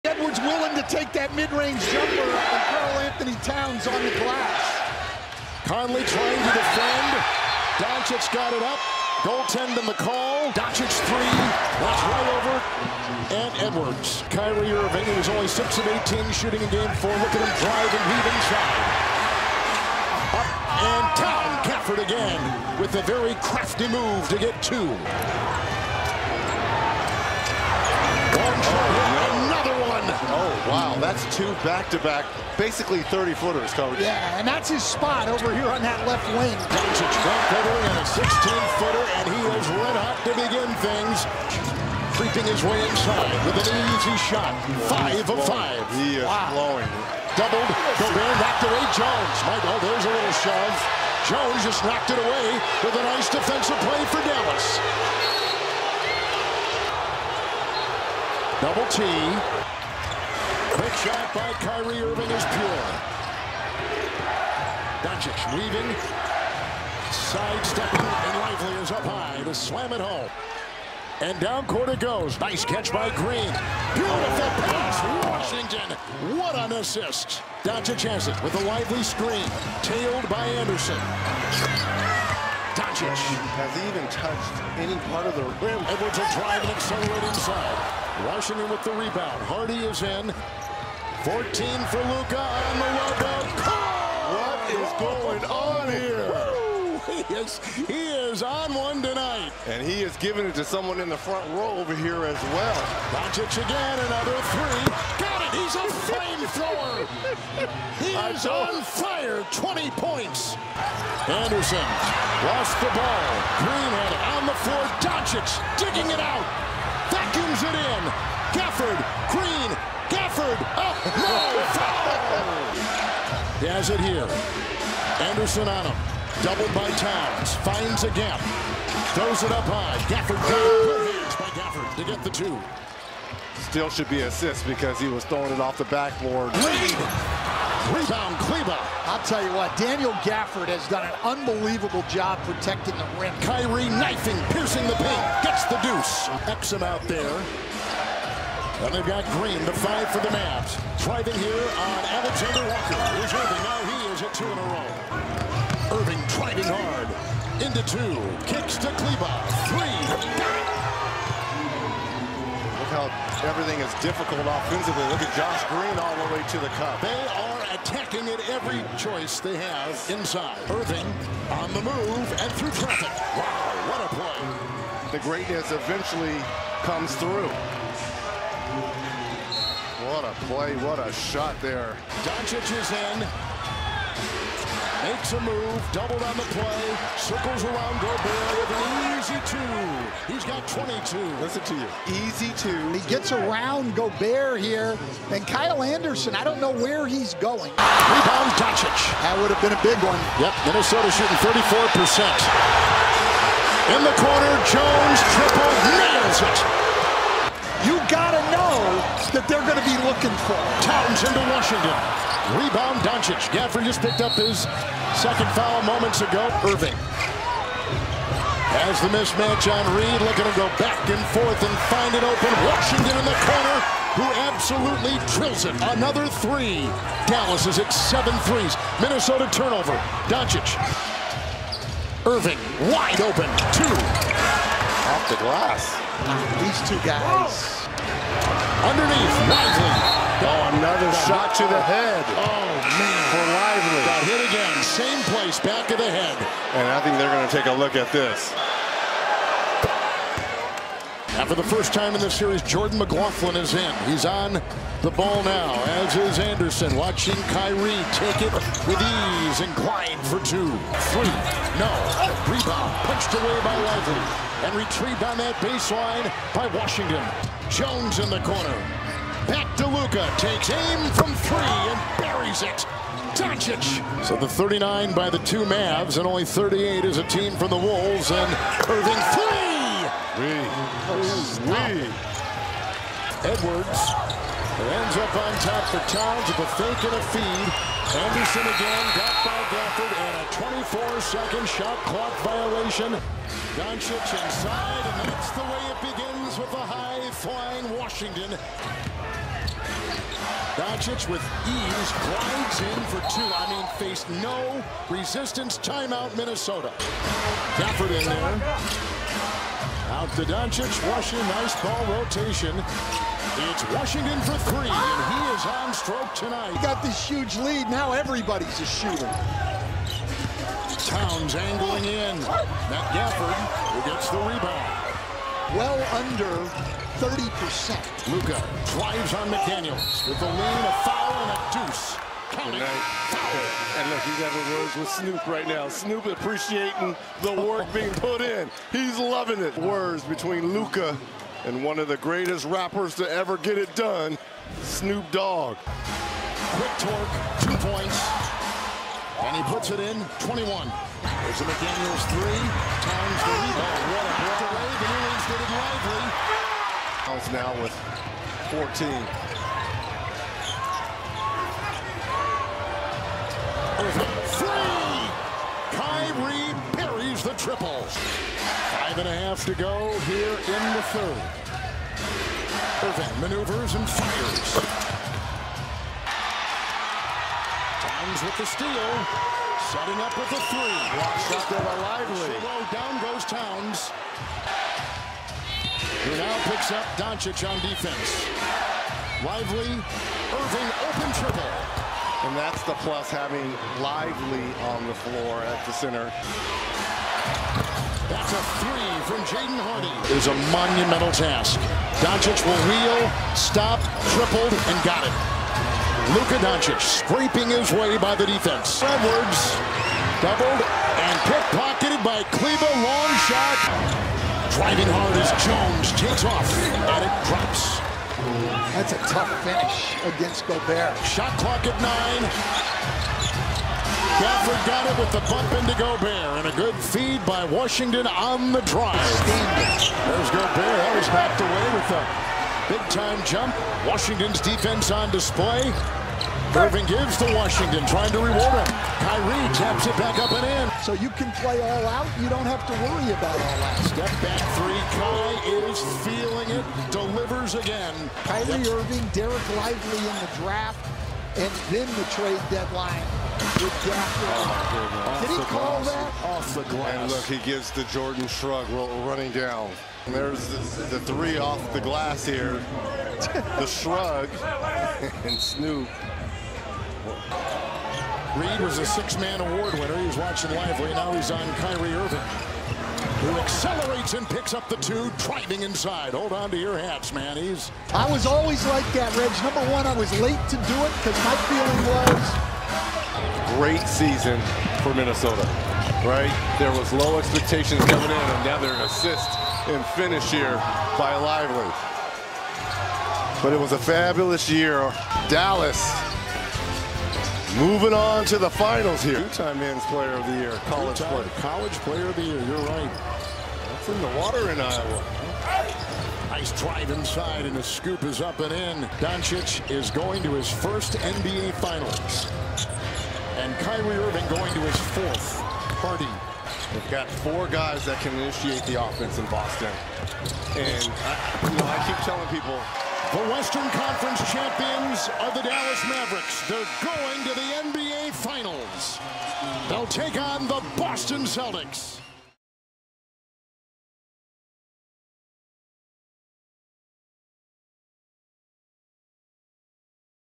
Dallas Mavericks Team Highlights vs the Timberwolves (2024 Playoffs WCF Game 5)
I appreciate all the support and feedback!